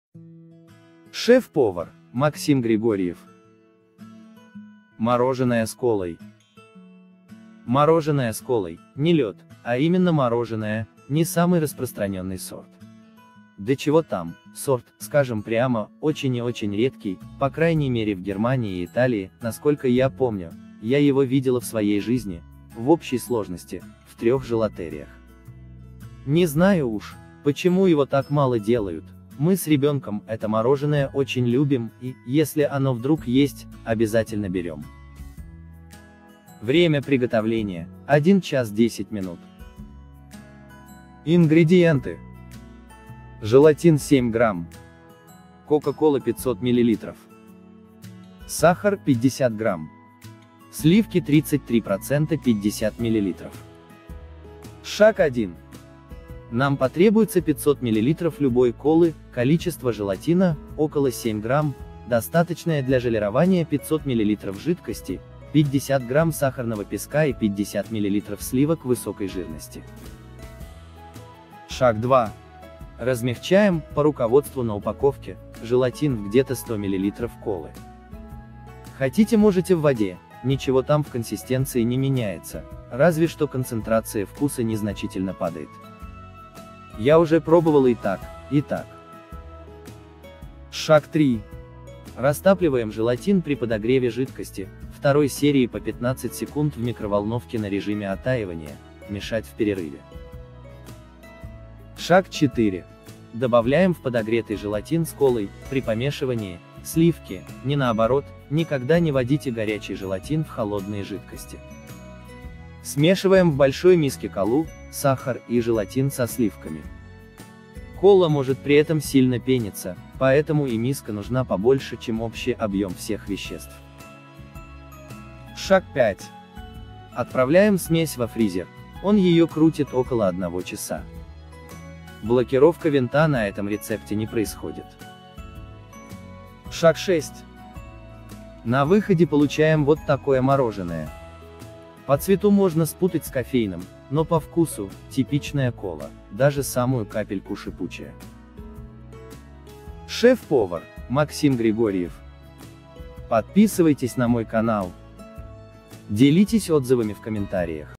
Мороженое с колой . Рецепт от шеф повара Максима Григорьева
Мороженое с колой  (не лёд, а именно мороженое) — не самый распространённый сорт. Да чего там, сорт, скажем прямо, очень и очень редкий, по крайней мере в Германии и Италии, на сколько я помню, я его видела в своей жизни, в общей сложности, в трёх желатериях. Не знаю уж, почему его так мало делают, мы с ребёнком это мороженое очень любим и, если оно вдруг есть, обязательно берём.